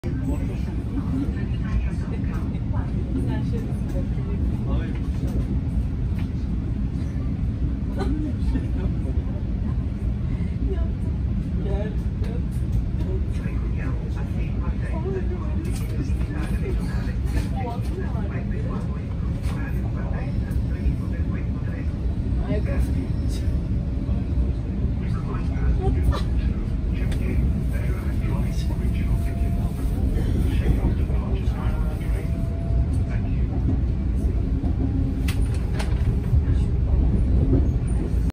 Passing shot of trains from Venice, Italy
Overcast skies, a newer train passes us on the way out of Venice, to reveal an unknown small island